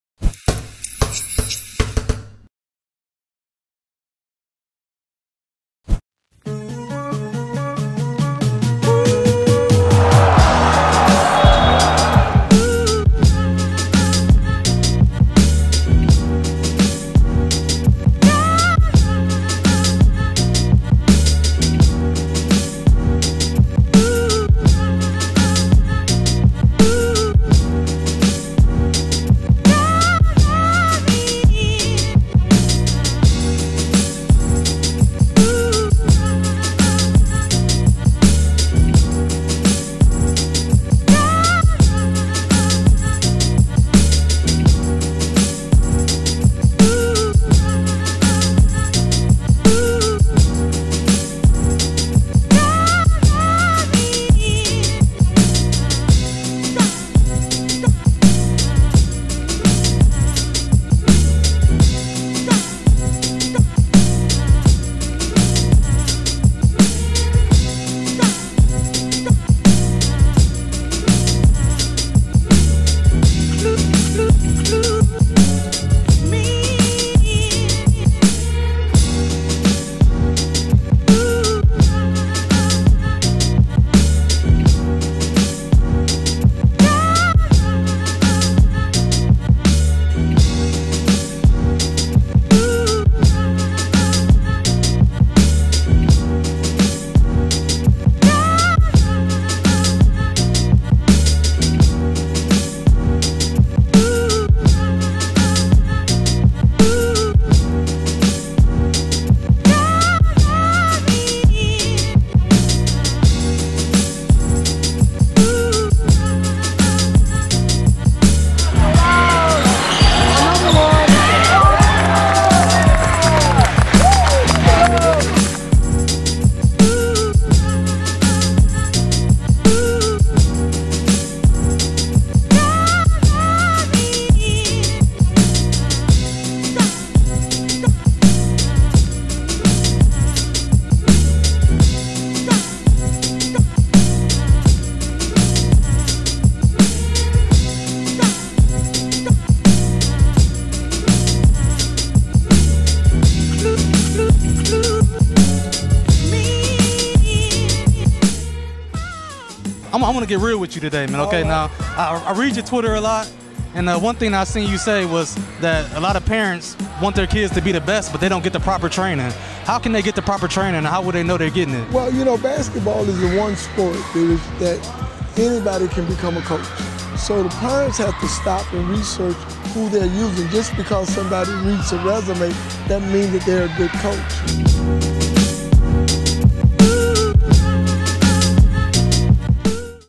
John Lucas All Star Wekeend 2016 | Zion Harmon, Kyree Walker and More! (Official Mix)
The 2016 John Lucas All Star Weekend in Houston, Tx was a weekend to remember. The event brought talent from all over the country for the chance to battle for rings. The Lucas All star weekend is becoming the National Championship of AAU Basketball.